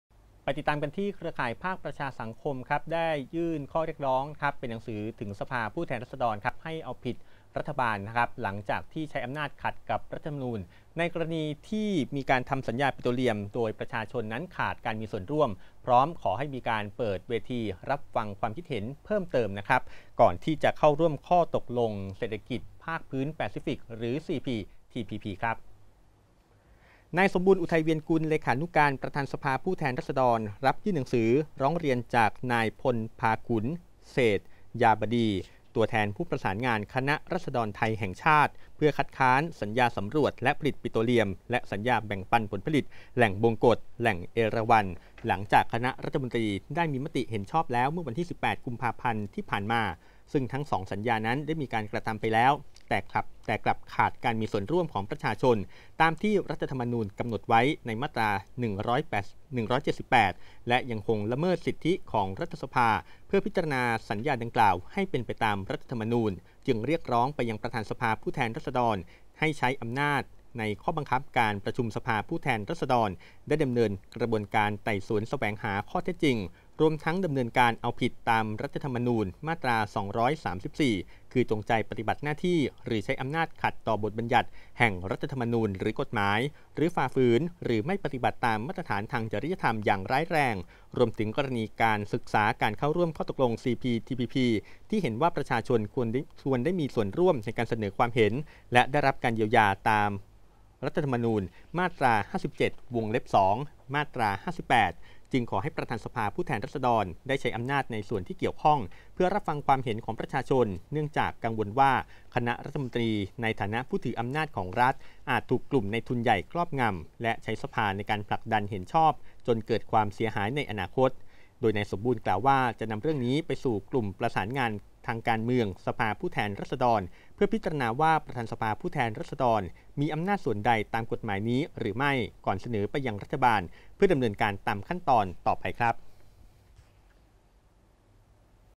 [NEWS]ร้องสภาฯ ให้ปชช.มีส่วนร่วมก่อนร่วมข้อตกลง CPTPP
ภาคประชาสังคม เรียกร้อง สภาฯ เอาผิดรัฐตาม    ม.234 ใช้อำนาจขัดรธน. กรณีทำสัญญาปิโตรเลียม ประชาชนไม่มีส่วนร่วม เตรียมเปิดเวทีรับฟังความเห็นเพิ่ม ก่อนเข้าร่วมข้อตกลง CPTPP